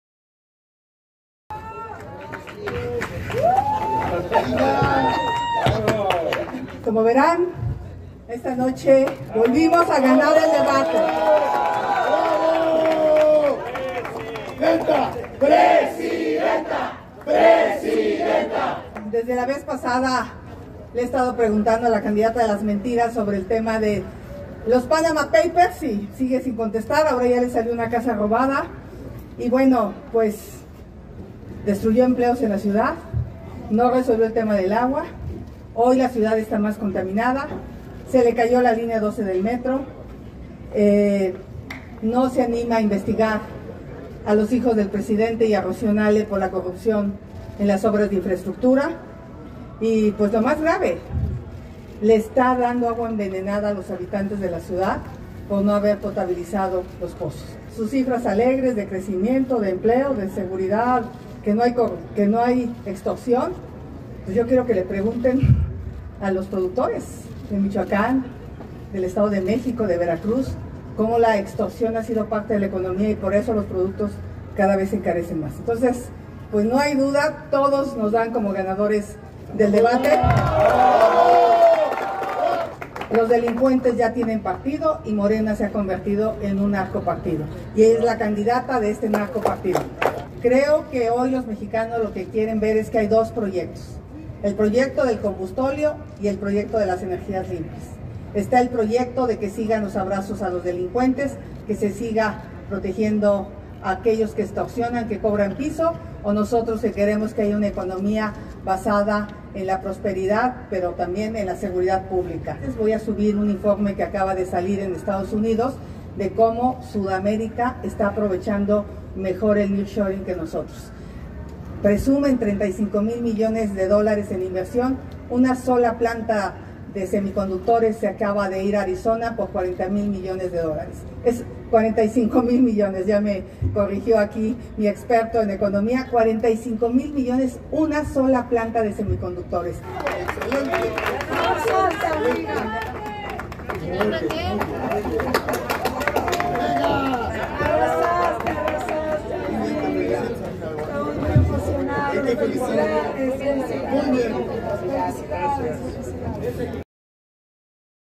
Xóchitl Gálvez también asegura haber ganado el debate
Estas son las palabras de Xóchitl Gálvez, candidata por el PRI, PAN y PRD, tras salir del debate presidencial. También asegura que ganó el debate, al igual que lo hace Claudia Sheinbaum.